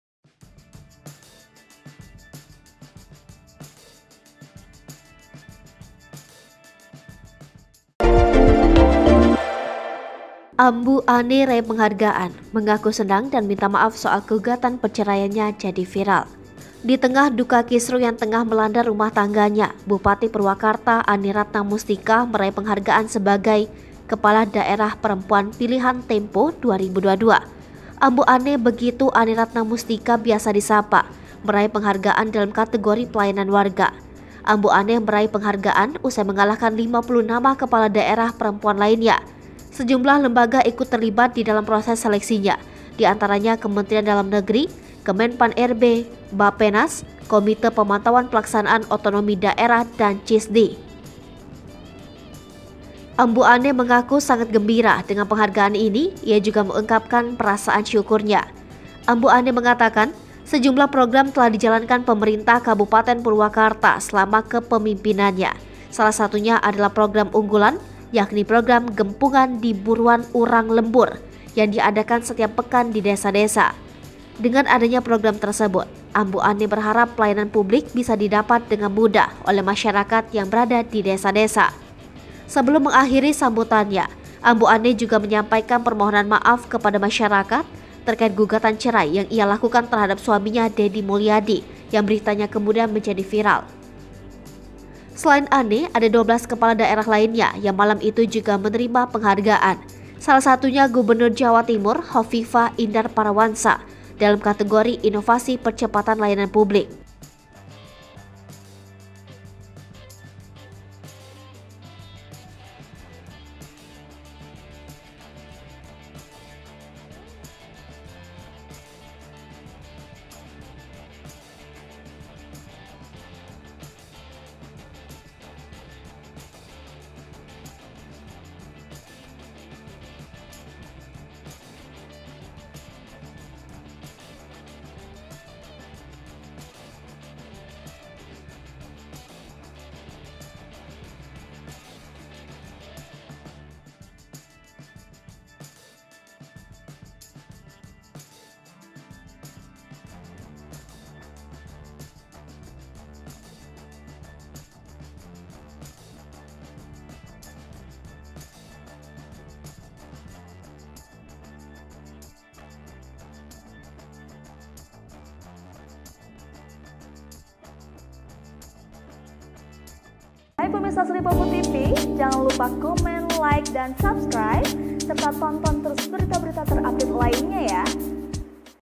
Ambu Anne Raih Penghargaan, Mengaku Senang dan Minta Maaf Soal Gugatan Perceraiannya Jadi viral
Di tengah duka kisruh yang tengah melanda rumah tangganya, Bupati Purwakarta, Anne Ratna Mustika, meraih penghargaan sebagai Kepala Daerah Perempuan Pilihan Tempo 2022.

Ambu Anne, begitu Anne Ratna Mustika biasa disapa, meraih penghargaan dalam kategori Pelayanan Warga.

Ambu Anne meraih penghargaan usai mengalahkan 50 nama kepala daerah perempuan lainnya.

Berita Selengkapnya :
Yuk lihat informasi terkini bersama kami, di:

Palembang.tribunnews.com

Editor: Sarah
Sumber: IG anneratna82, Diskominfo Purwakarta